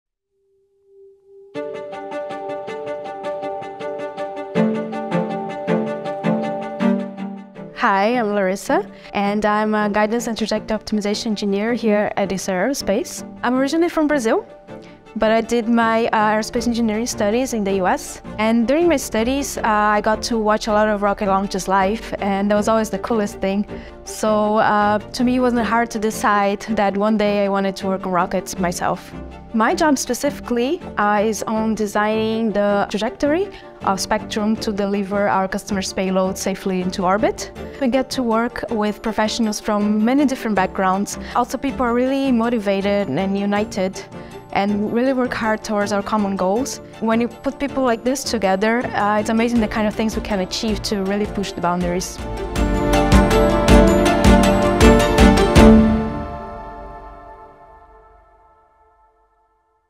Meet our team: Larissa Balestrero Machado, Guidance & Trajectory Optimization Engineer
Meet Larissa, Guidance & Trajectory Optimization Engineer at Isar Aerospace in Ottobrunn, Germany. Originally coming from Brazil, Larissa studied in the US where she developed a fascination for space from watching rocket launches.

After that, it wasn't hard for her to decide that one day, she wanted to work on rockets and in spaceflight herself. This fascination still drives her, but even more so working with a great team: "I get to work with professionals from many different backgrounds, and people are really motivated, united and work hard towards our common goals. When you put people like this together, it's amazing what you can achieve to push the boundaries!"